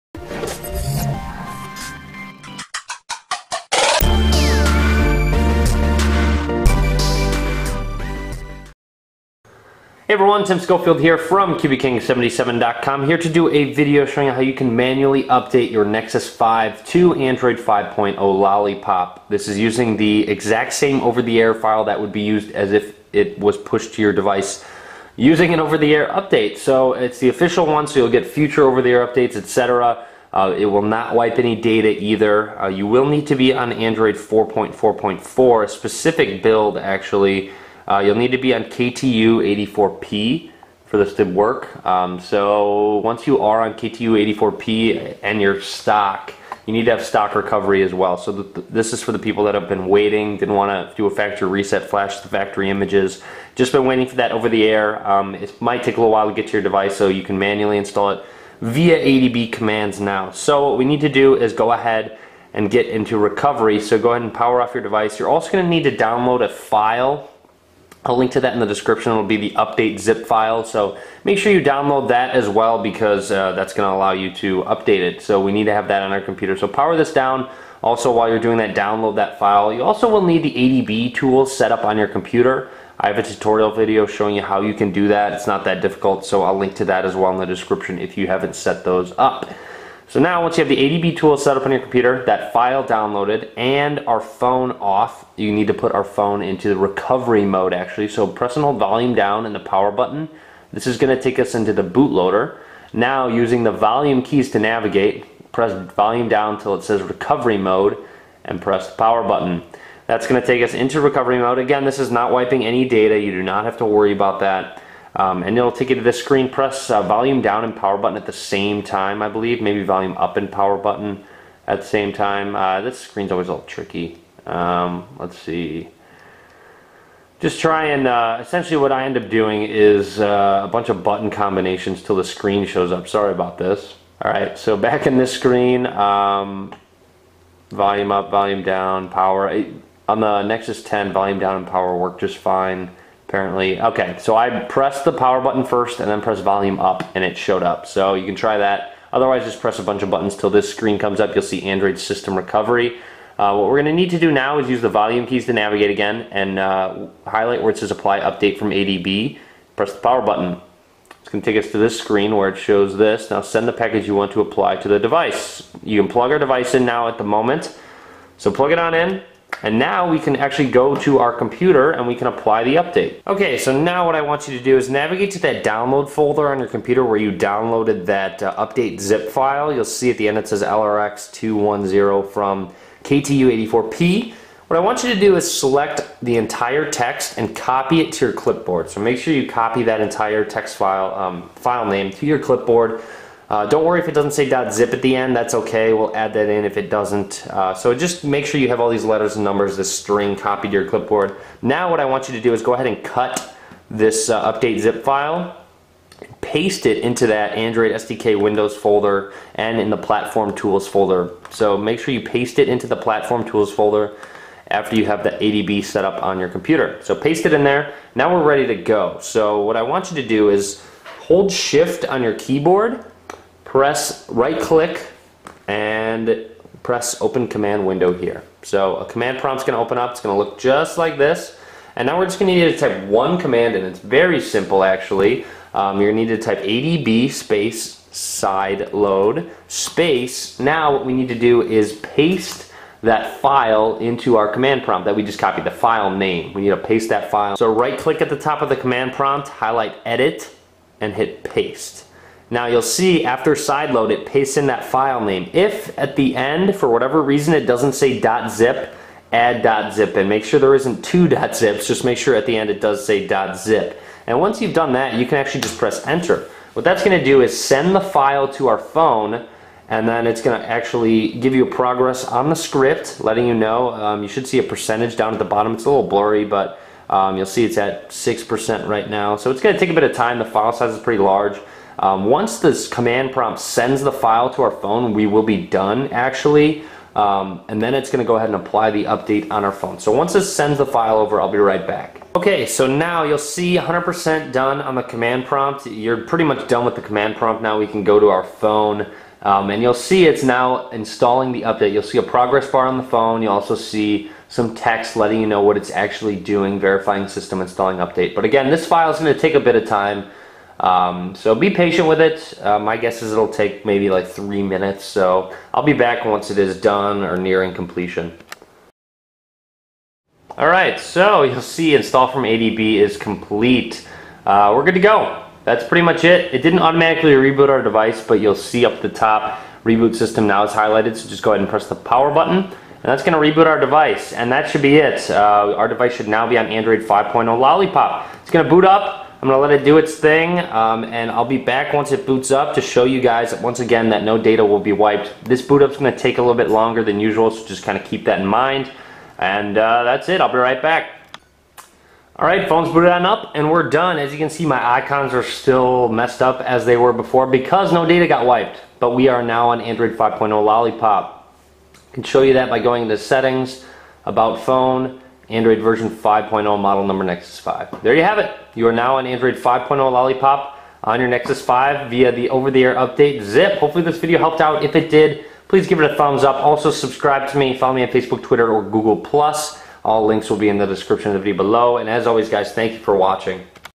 Manually Update Nexus 5 to Android 5.0 Lollipop
For those that have been waiting for the OTA update, you can now manually install it! Just follow the video and you will have official Android 5.0 Lollipop on your Nexus 5.

Enable developer options on phone, turn usb debugging on and plug into PC